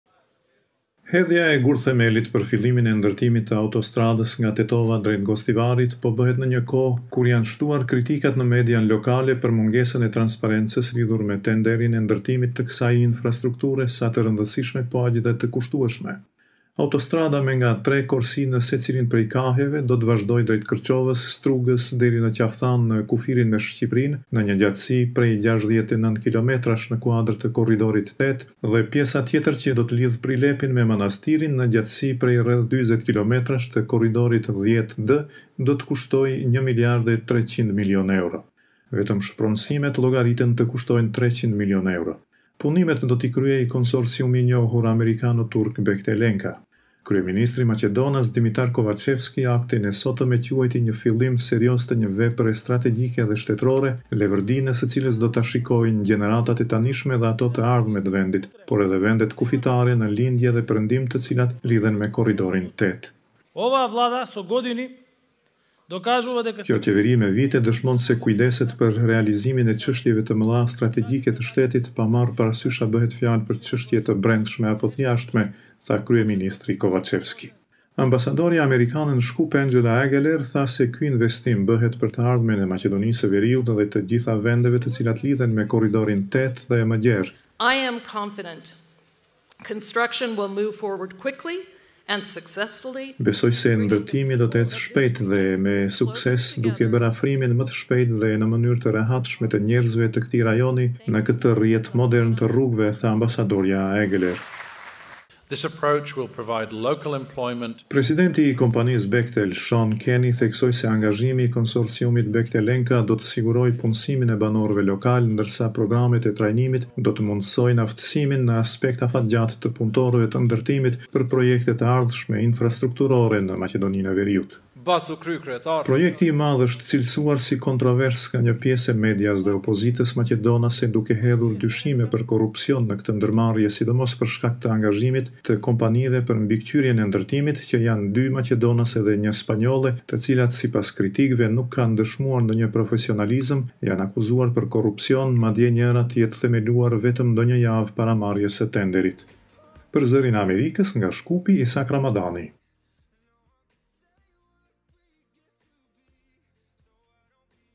Maqedoni e Veriut, përurohet nisja e punimeve në Korridorin 8
Sot u përurua nisja e punimeve në autostradën nga Tetova për në Gostivar, si pjesë e Korridorit 8, ndërtimin e së cilës do ta bëjë konsorciumi i njohur Bechtel-Enka. Autostrada do të vazhdojë deri në Strugë dhe Qafëthanë, ndërsa pjesa tjetër nga Prilepi në Manastir bëhet në kuadër të Korridorit 10 d